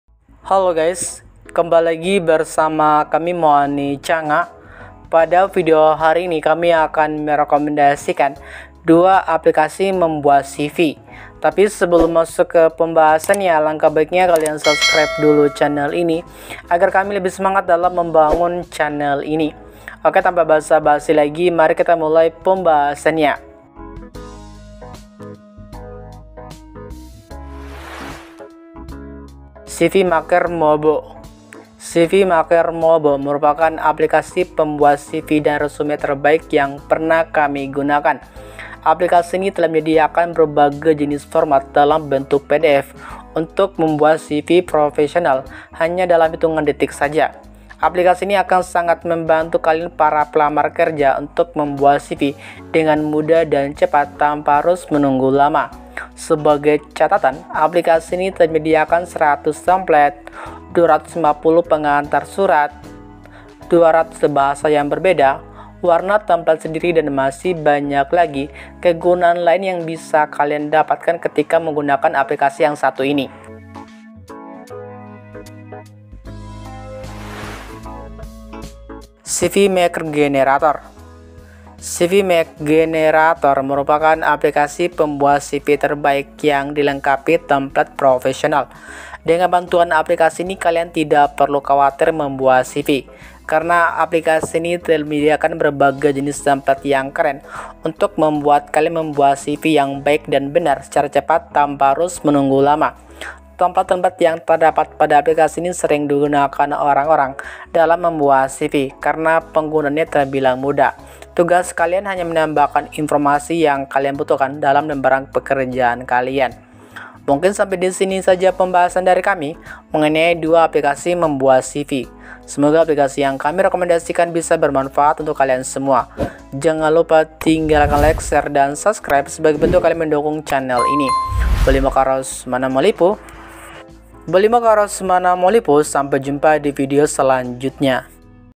2 Aplikasi Pembuat CV Lamaran Kerja Tembus HRD
Ingin membuat CV lamaran kerja yang tembus HRD namun bingung bagaimana caranya? Yuk, simak video ini dan temukan 2 aplikasi pembuat CV lamaran kerja terbaik dan paling efektif untuk membuat CV yang menarik dan tembus HRD. Tidak perlu khawatir karena kami akan menjelaskan dengan detail cara menggunakannya sehingga lebih mudah untuk memaksimalkan fitur-fitur yang ada dalam aplikasi tersebut.

Dengan video ini, kamu dapat menyiapkan CV lamaran kerja yang profesional dan membuka peluang lebih besar untuk ikut seleksi interview. Jangan lewatkan informasi berharga ini dan mulailah mempersiapkan diri untuk karier yang lebih baik